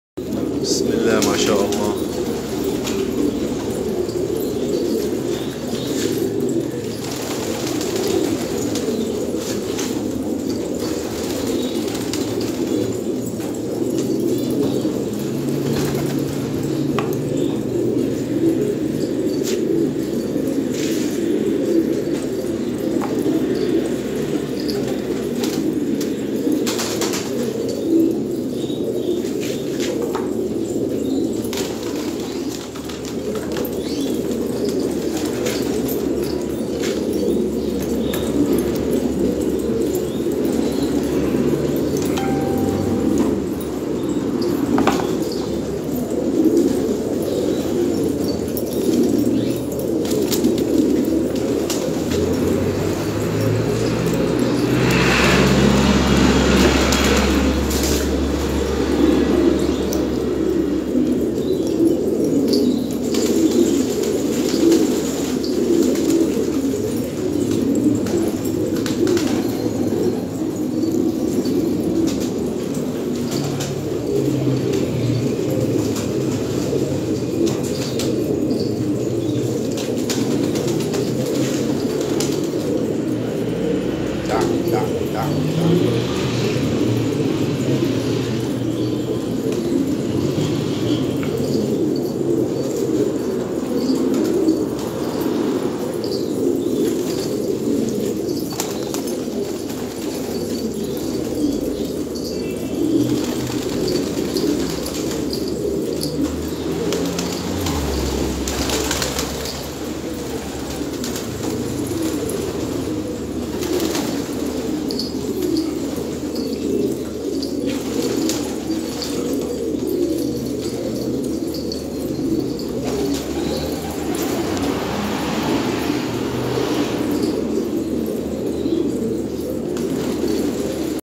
حمام للبيع 100طير عرسال لبنان المعلومات أسفل الفيديو في صندوق الوصف 👇👇 sale pigeons
Sale pigeons

منقول عن المصدر:
١٠٠طير للبيع موجودة بعرسال حقن مليون ٧١٦٨٤١٣٣

لمحبين كش الحمام انضمو لقناتنا الاساسية على اليوتيوب